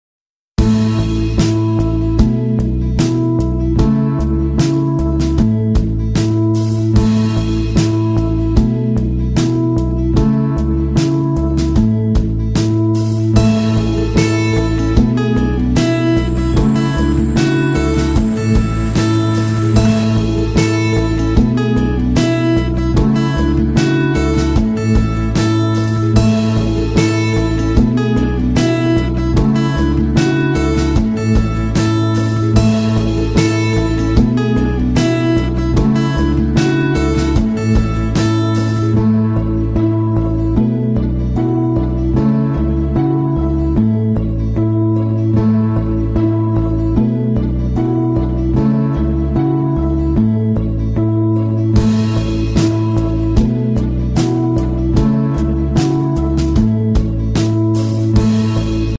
Daily Routine | Gardening Vlog | Gardening is my passion
So Viewers, this is my Gardening activity today where I also prepared Pot for Planting Elaichi - Cardamom seeds!
In the next video, I am going to show you how to plant Cardamom seeds!